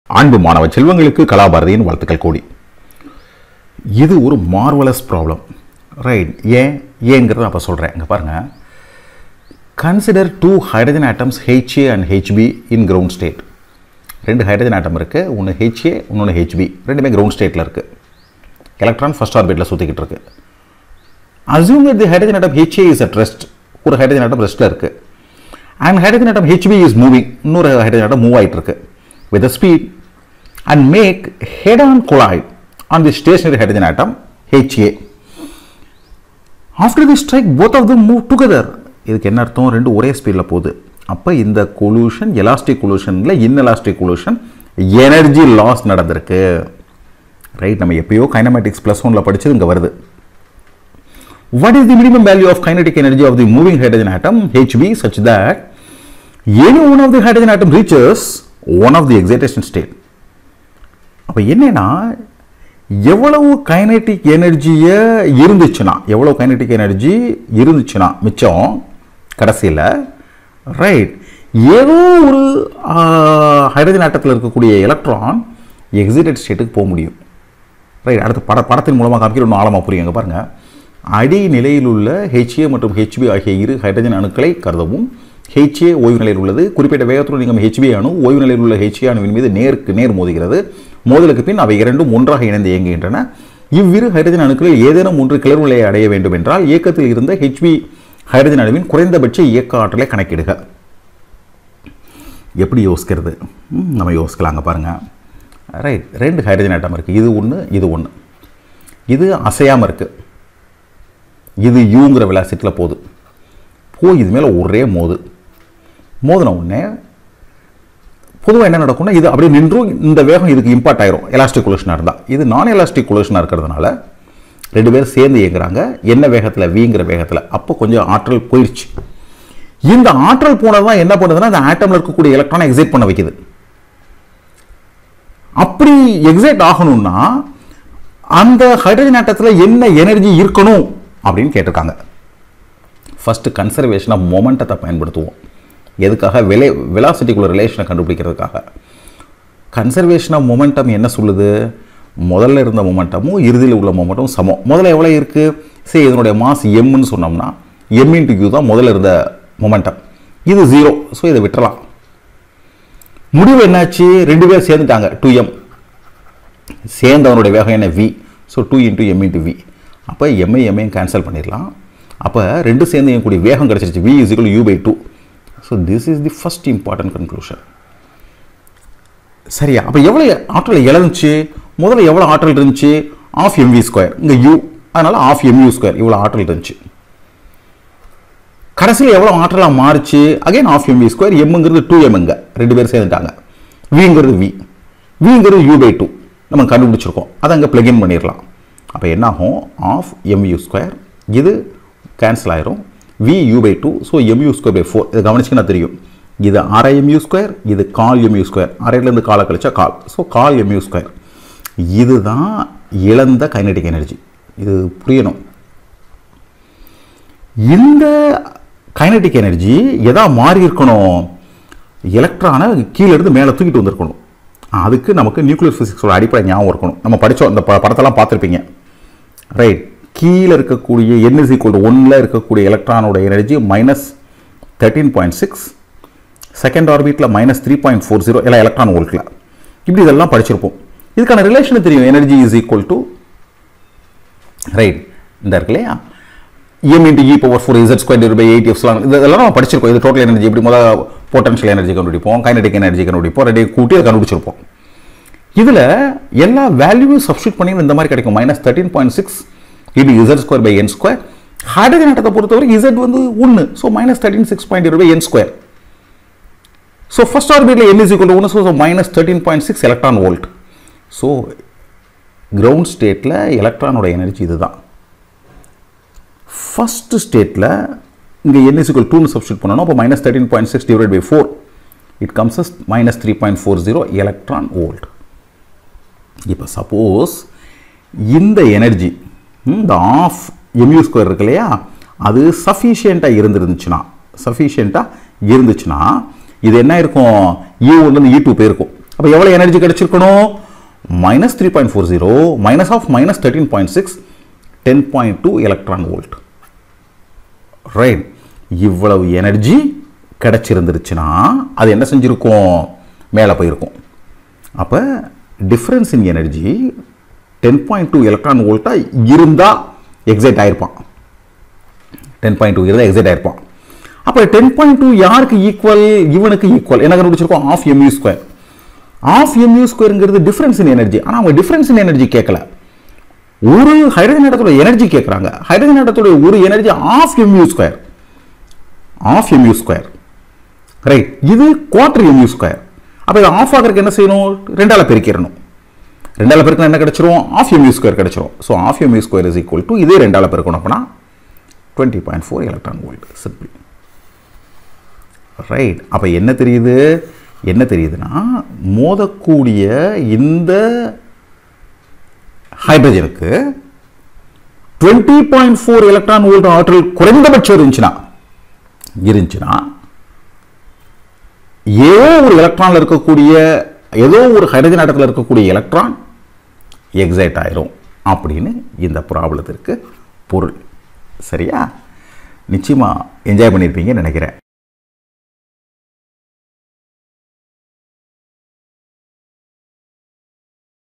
Exercise 1 | Physics | Atomic and nuclear physics | Class 12 | Chapter 8 in Tamil
Consider two hydrogen atoms HA and HB in ground state. Assume that hydrogen atom HA is at rest and hydrogen atom HB is moving with a speed and make head-on collide on the stationary hydrogen atom HA. After the strike, both of them move together. What is minimum value of the kinetic energy of the moving hydrogen atom HB, such that any one of the hydrogen atoms reaches one of the excitation state.